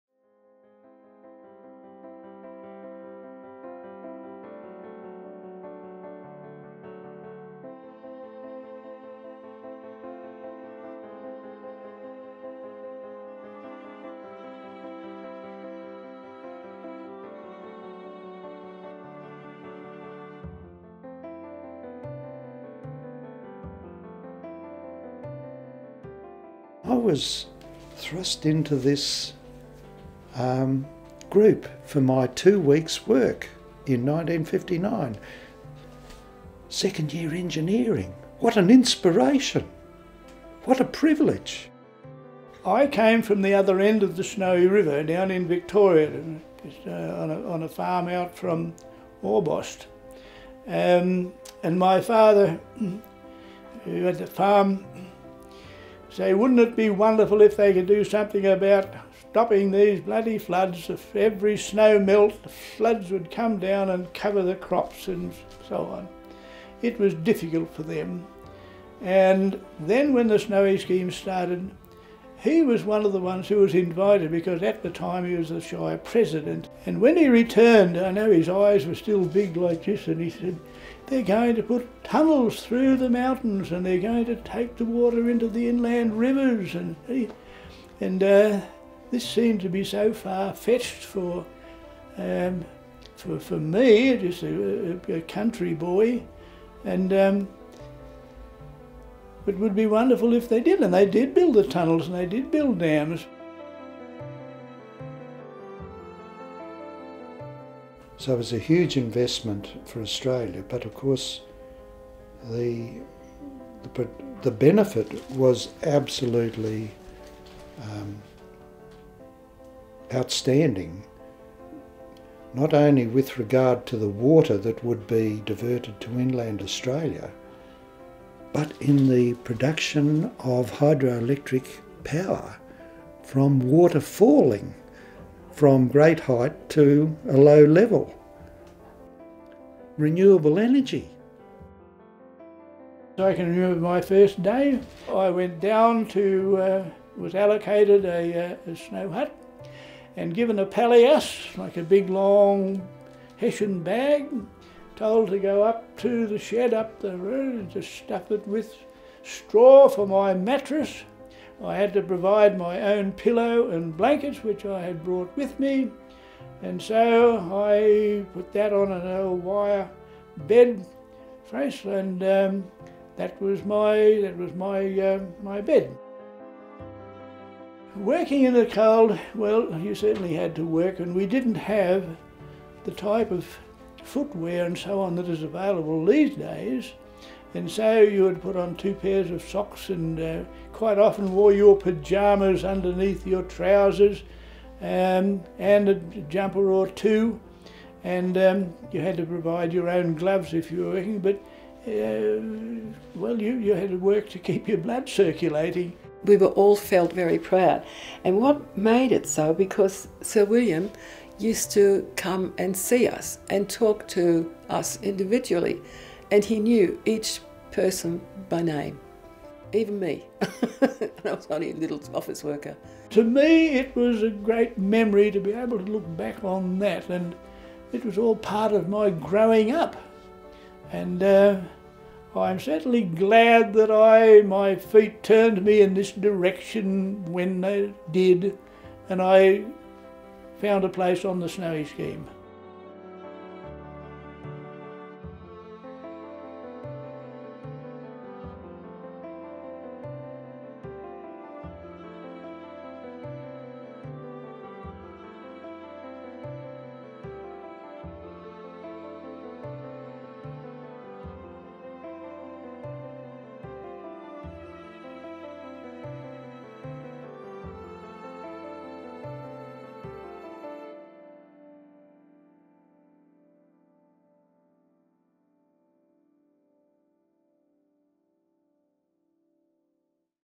Australia’s greatest engineering feat – the National Heritage Listed Snowy Mountains Scheme
The Snowy Mountains Scheme is the largest public works engineering scheme ever undertaken in Australia. The scheme is nationally significant for its engineering success and as a symbol of Australian achievement. The Snowy Mountains Scheme was placed on the National Heritage List on 14 October 2016.